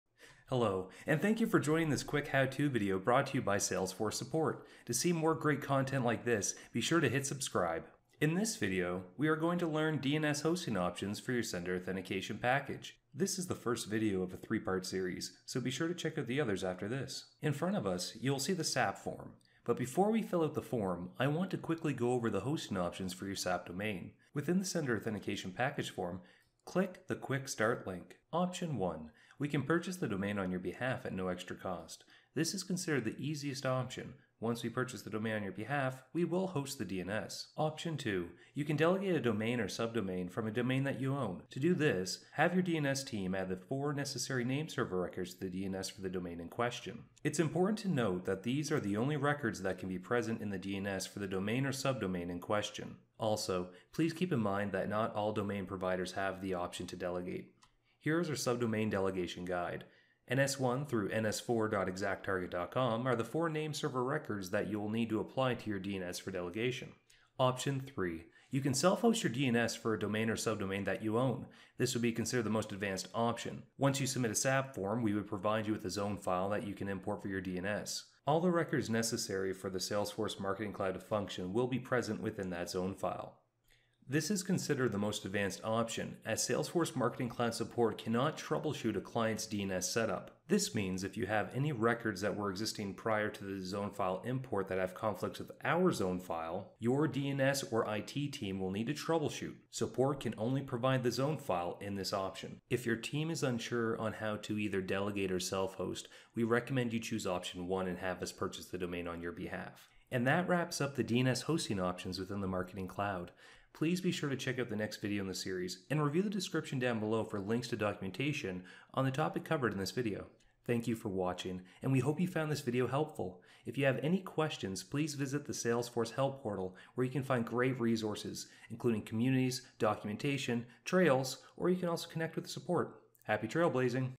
Understand DNS Hosting for Your Sender Authentication Package | Part 1 | Salesforce Marketing
This video walks through the 3 DNS hosting options for Marketing Cloud - Email Studio. For more information on: